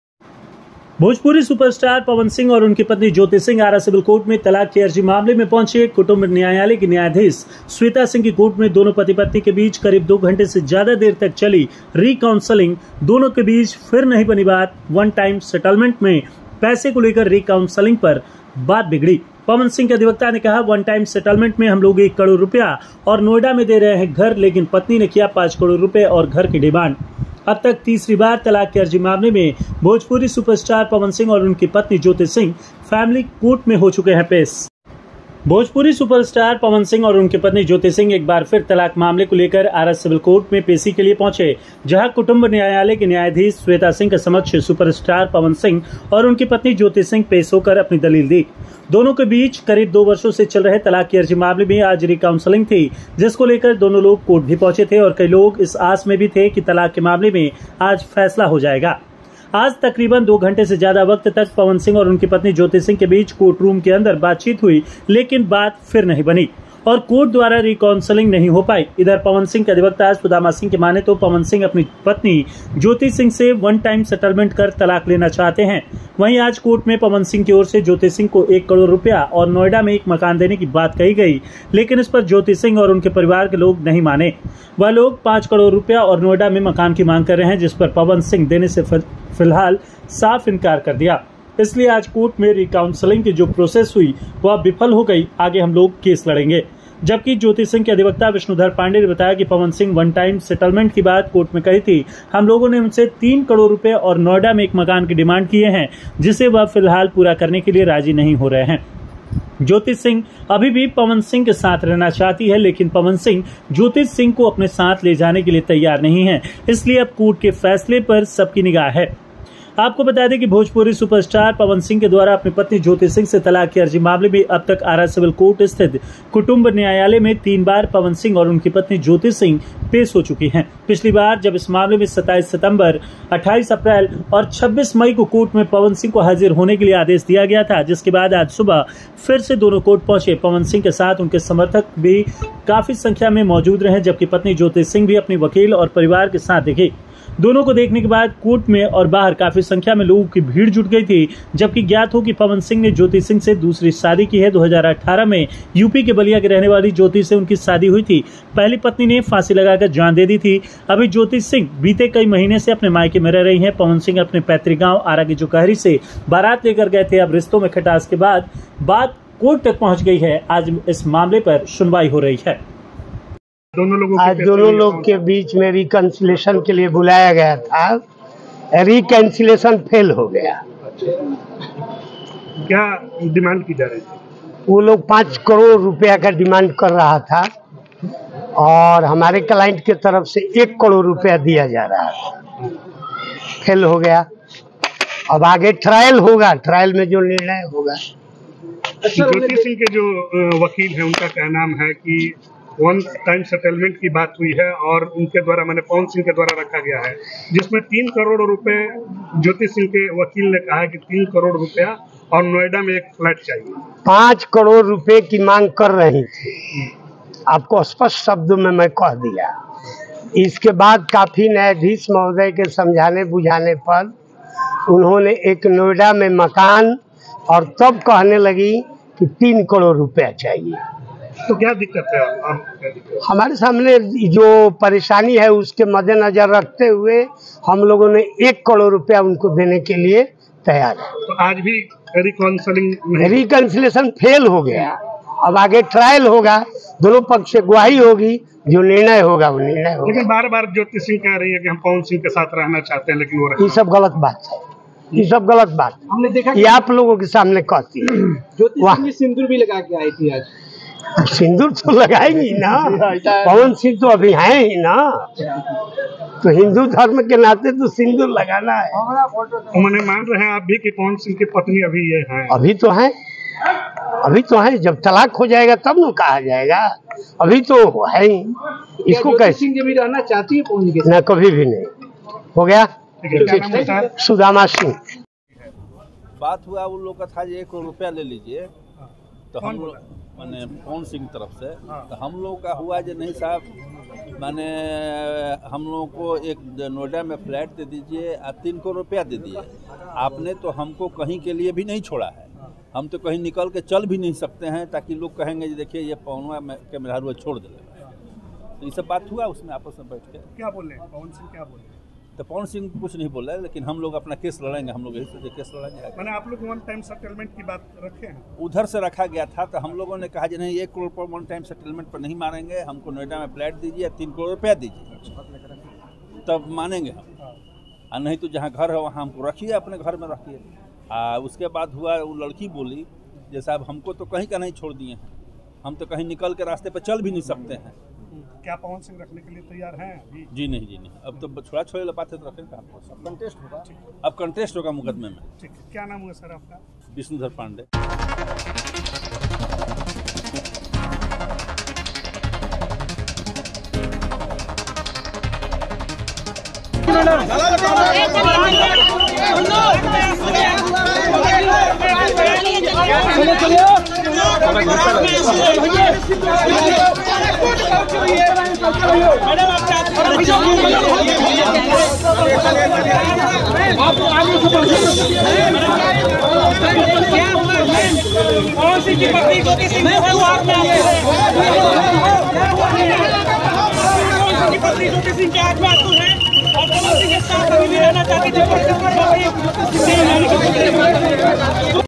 Powerstar Pawan Singh और पत्नी Jyoti Singh के बीच फिर नहीं बनी बात, वन टाइम सेटलमेंट पर फंसा पेंच
पवन सिंह तलाक प्रकरण: भोजपुरी सुपरस्टार पवन सिंह और उनकी पत्नी ज्योति सिंह के बीच फिर नहीं बनी बात, वन टाइम सेटलमेंट पर फंसा पेंच